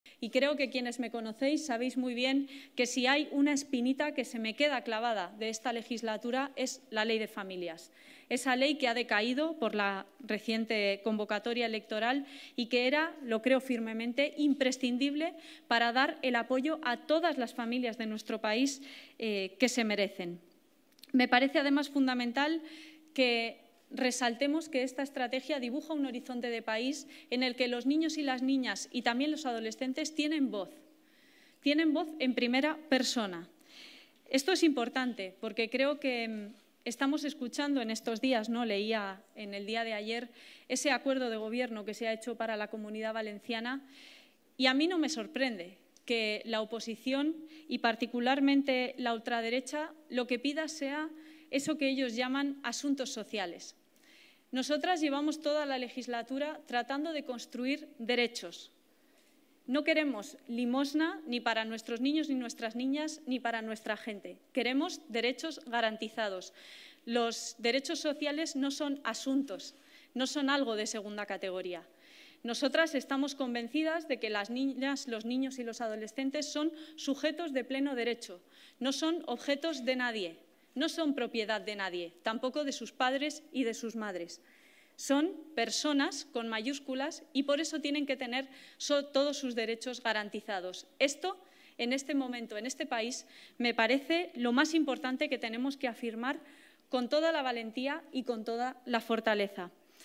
Belarra alerta de que los derechos de la infancia se conviertan en una "limosna" con PP y Vox
La ministra de Derechos Sociales y Agenda 2030, Ione Belarra, ha alertado del riesgo de que los derechos sociales y los derechos de la infancia se conviertan en una "limosna" con un gobierno de PP y Vox como el que se ha formado en la Comunidad Valenciana.